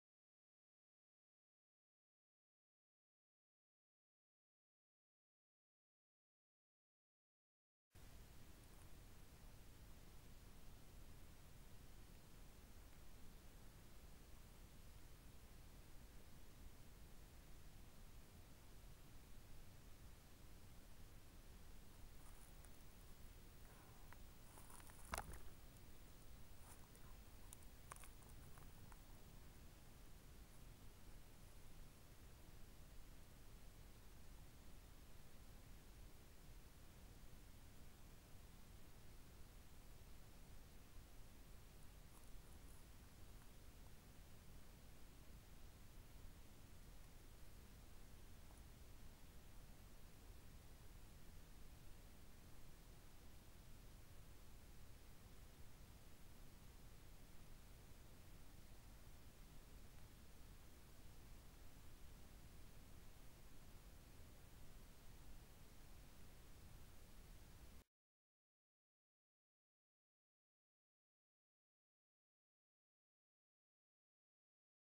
Tapir - Tapirus terrestris
Tapir's movie at Lauraceas State Park in Atlantic Forest - Paraná, Brazil.